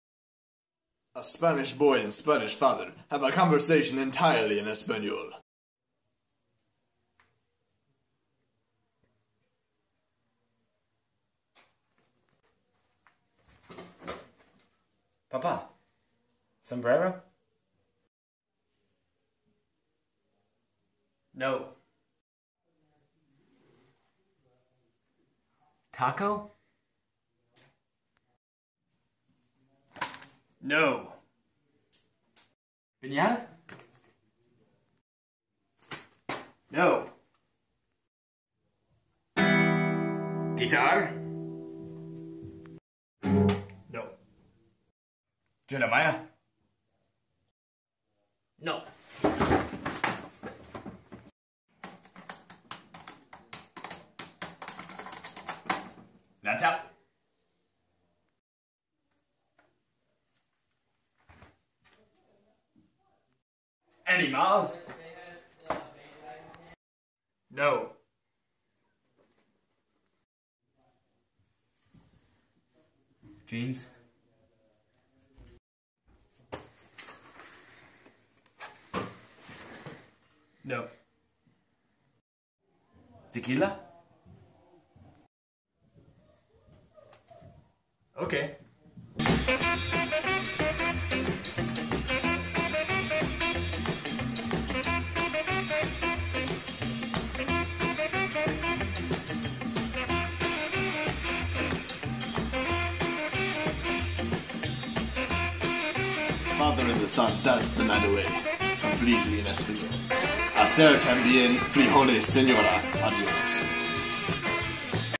Completely in Español
A father and a son have a conversation entirely in Español